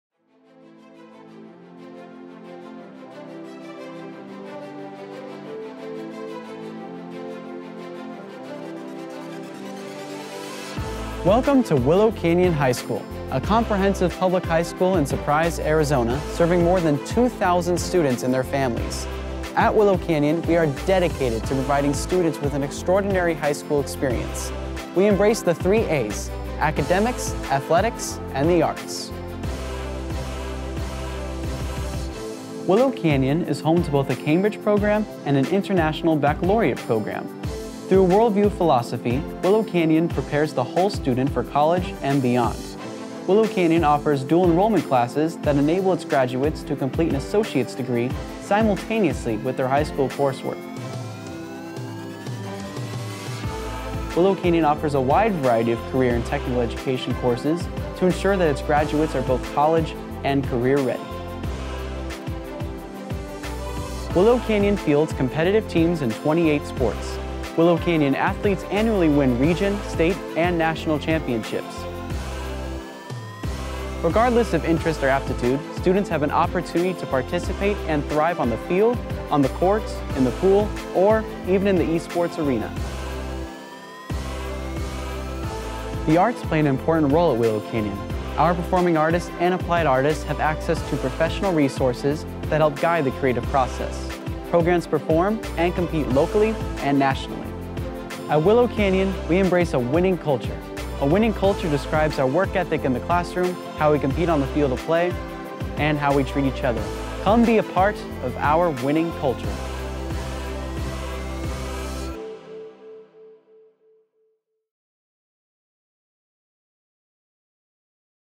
Welcome to Willow Canyon High School
Willow Canyon High School is a 9-12 school that serves 2,000 students in Surprise and the surrounding area. Willow Canyon is a comprehensive high school that offers a variety of programs to meet students’ interests. Within those programs, Willow Canyon is able to challenge students of all abilities to prepare them for success. Willow Canyon is committed to the development of a Winning Culture which applies to our academics, our arts, our athletics, how we treat each other, and every facet of our school.